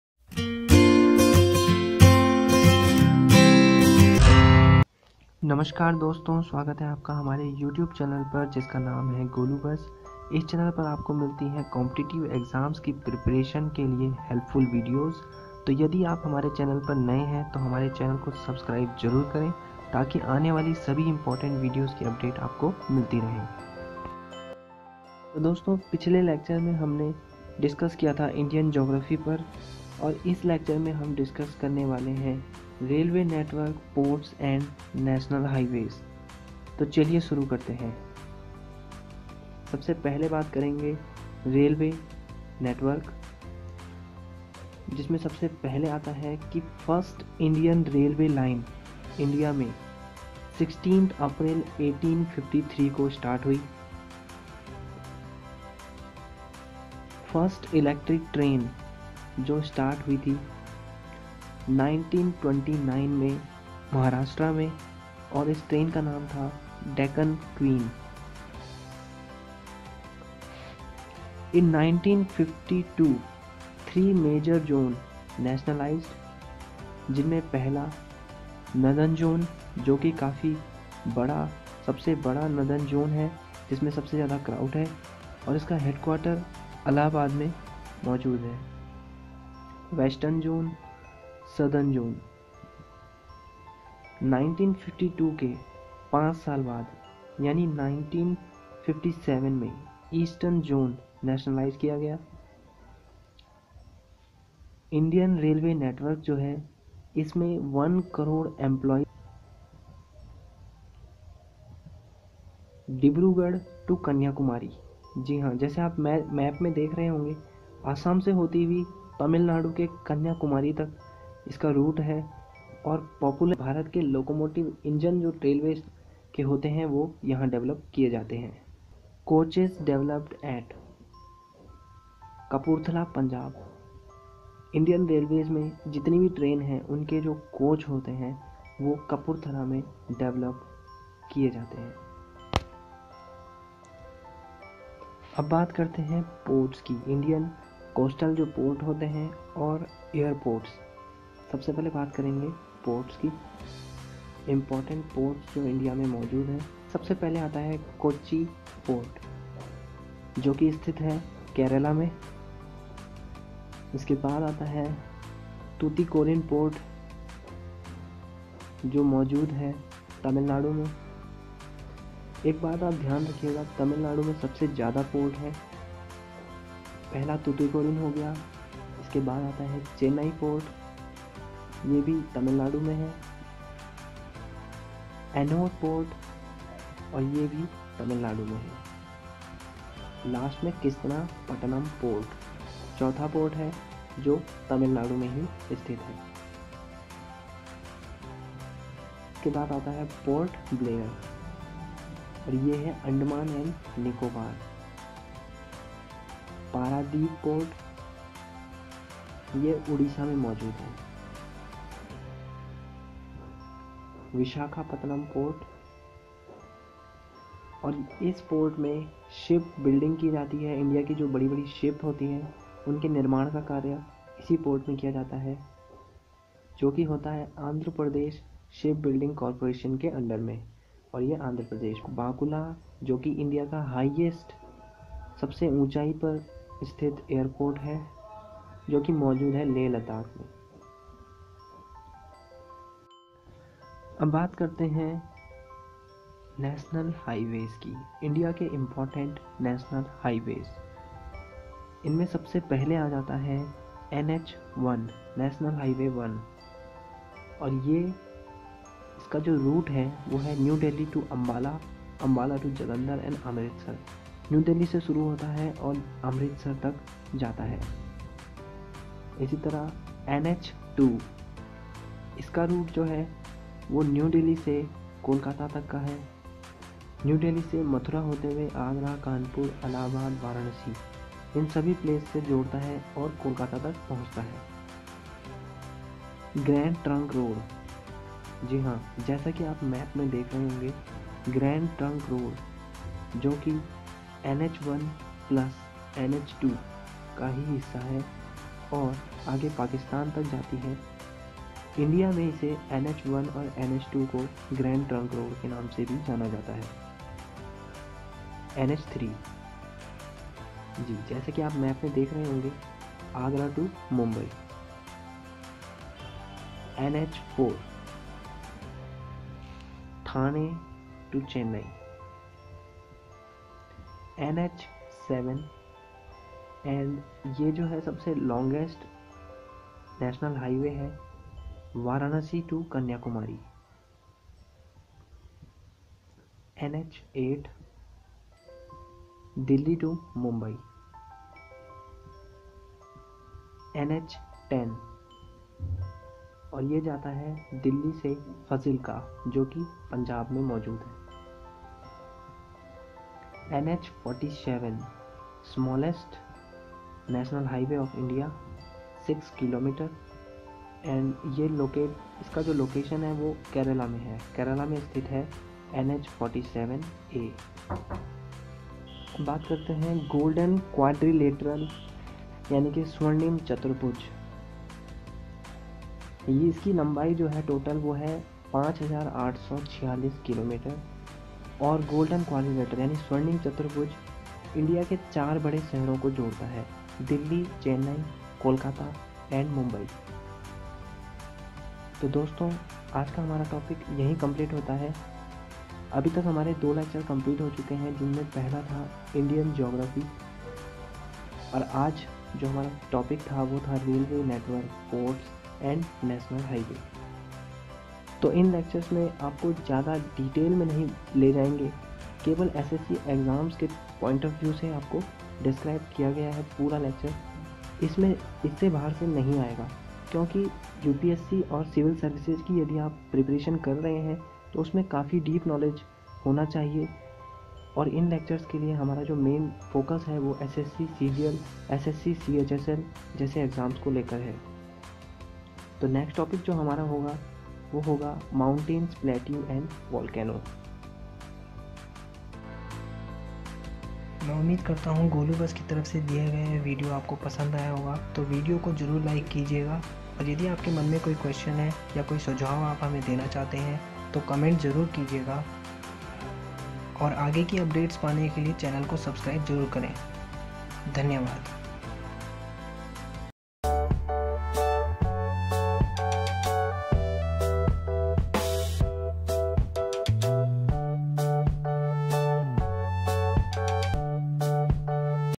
Geography Lecture 2 - Railway Network, Ports And National Highways | GS For SSC CGL Exam 2018
General Study (Geography) For SSC CGL Exam 2017 Preparation Online Classes..Geography Lecture 2  - Railway Network, Important Ports And National Highways.